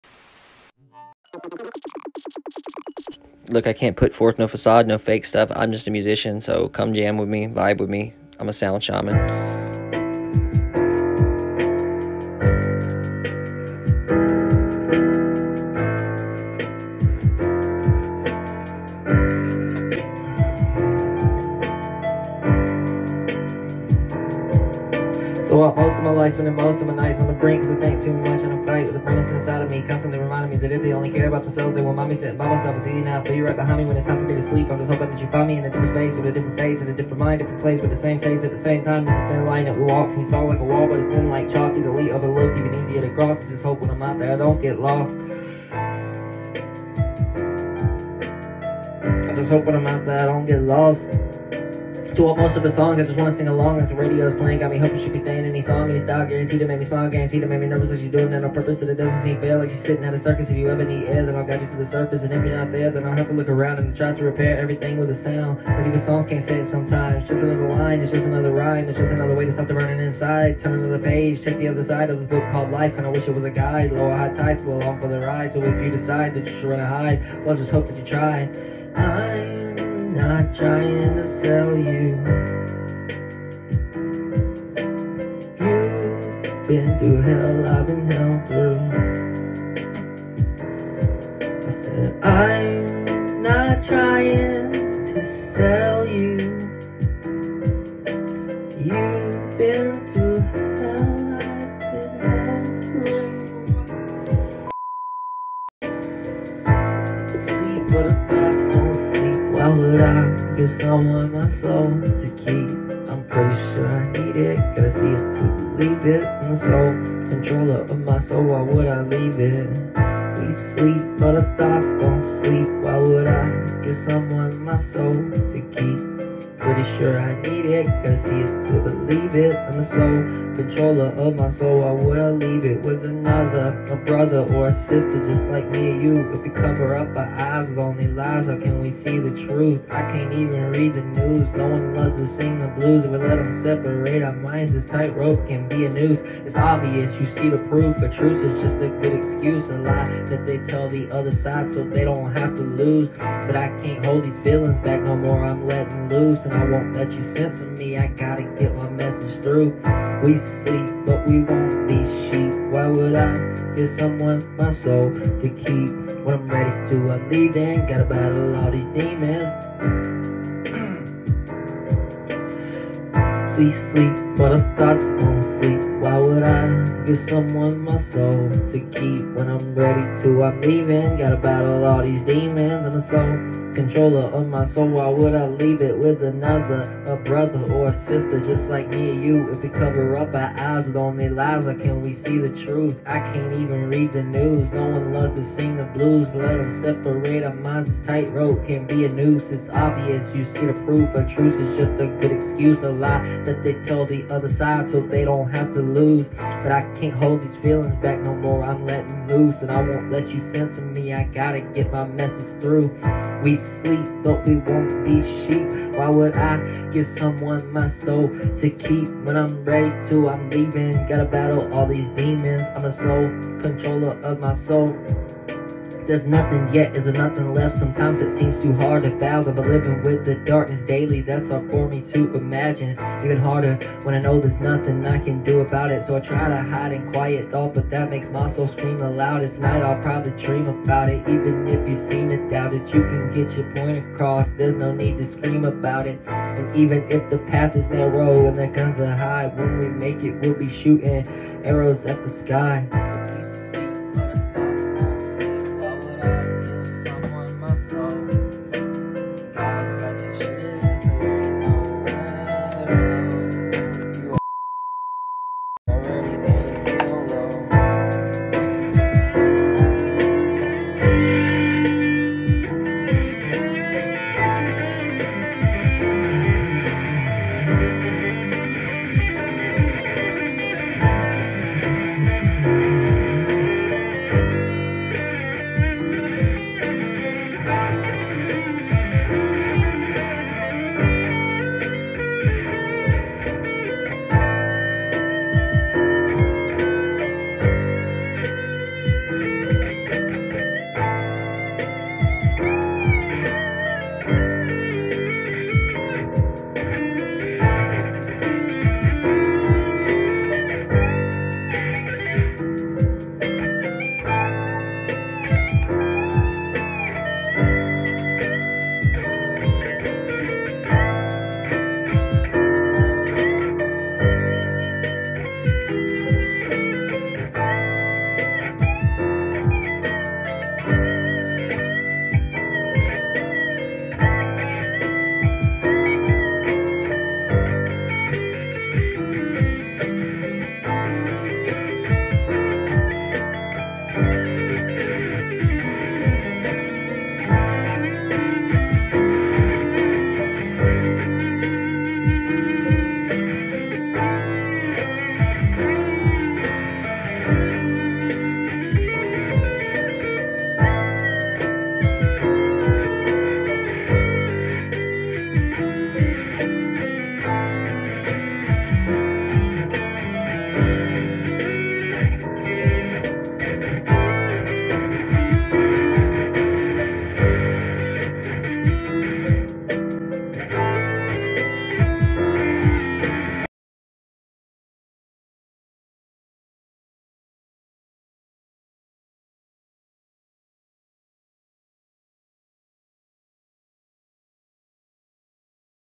How to rap and rock like the pros
Me jamming to my own stuff